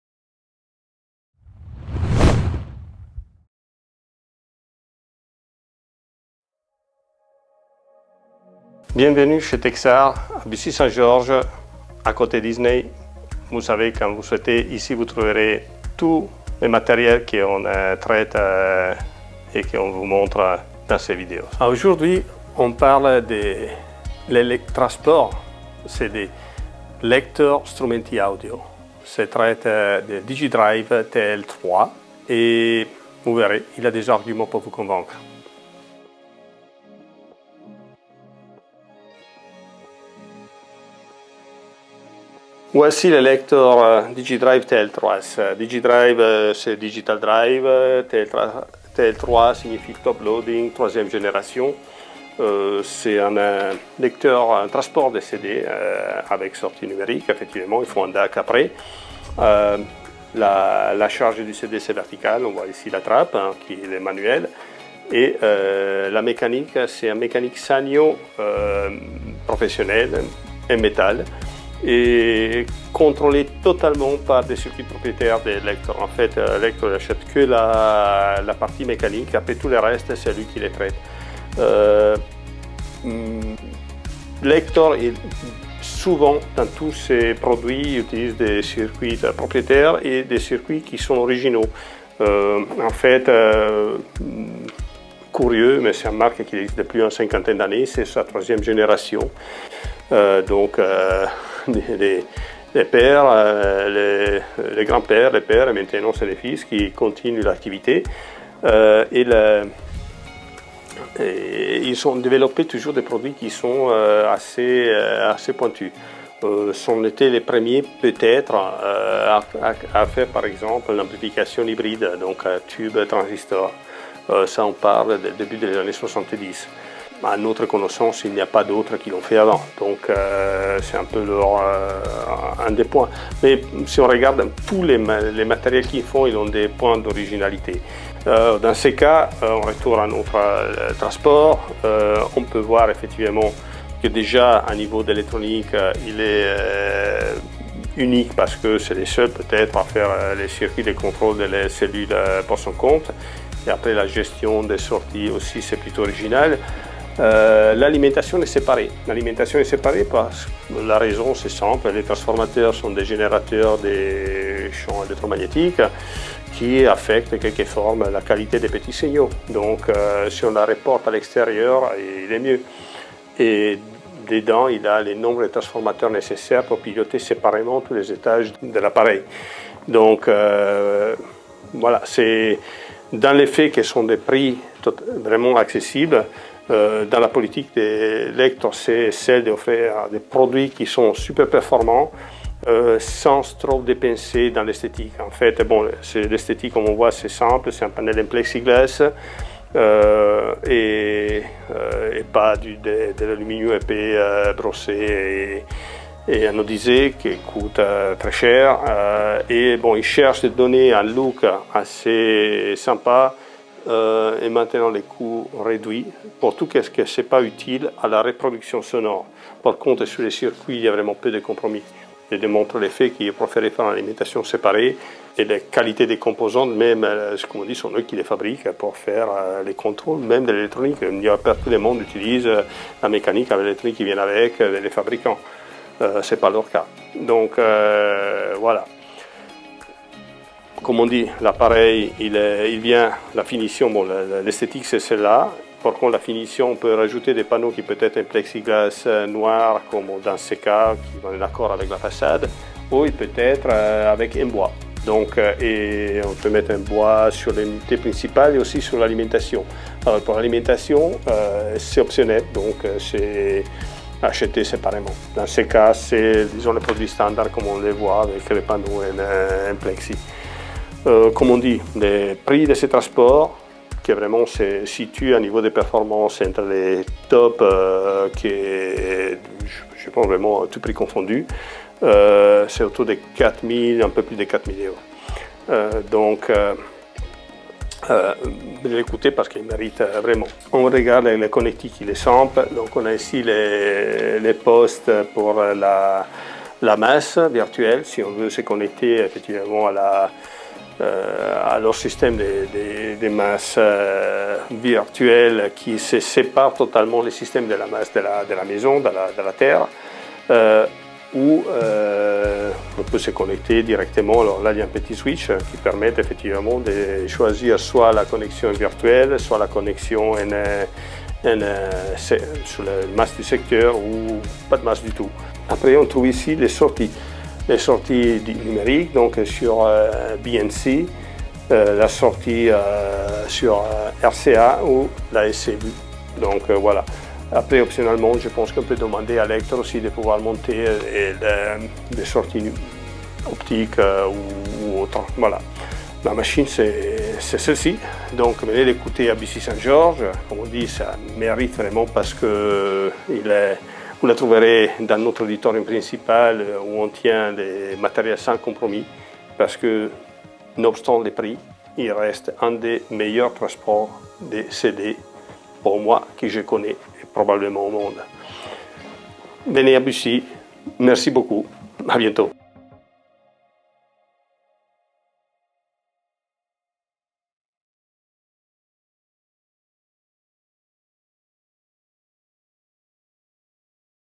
Présentation du Digidrive TL3 du lector Strumenti Audio par Tecsart.
Tecsart vous présente le Digidrive TL3 de Lector Strumenti Audio.

Le transport de référence Digidrive Top-Loading combine de nombreuses années d’expérience en conception et en physique appliquée à l'audio haut de gamme avec un mécanisme pur, une électronique de pointe, des détails audiophiles et tous les équipements nécessaires pour ouvrir une nouvelle dimension dans le plaisir musical

Un matériau spécial comme le boîtier en aluminium, les transformateurs numériques de sortie et la section électronique à servomoteur de haute qualité assurent un équilibre parfait pour des performances authentiques.

Un véritable lecteur de CD à chargement par le haut n'a pas de stabilisateur de disque central intégré au couvercle, mais le stabilisateur de disque doit être libre pour une insertion manuelle.

La grande façade monobloc en plexiglas, le corps monobloc en aluminium et surtout la grande structure interne en laiton garantissent une excellente rigidité et stabilité mécanique.